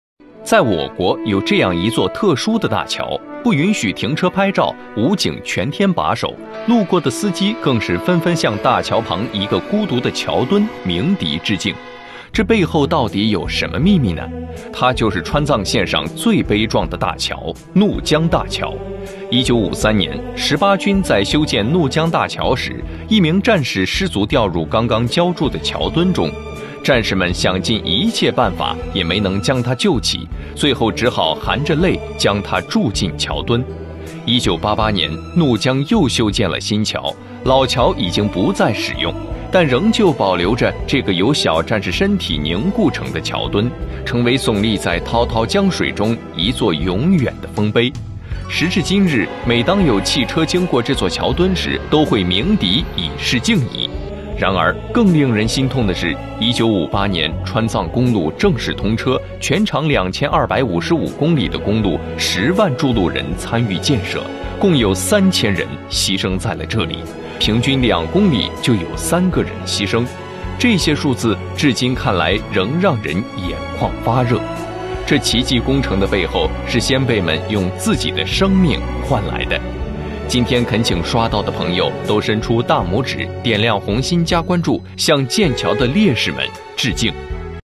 318必经之路#怒江大桥，经过这里的车辆都会鸣笛致敬，背后的英雄故事，让人落泪！The Must-Pass Route of 318 # Nujiang Bridge.
318必经之路#怒江大桥，经过这里的车辆都会鸣笛致敬，背后的英雄故事，让人落泪！The Must-Pass Route of 318 # Nujiang Bridge.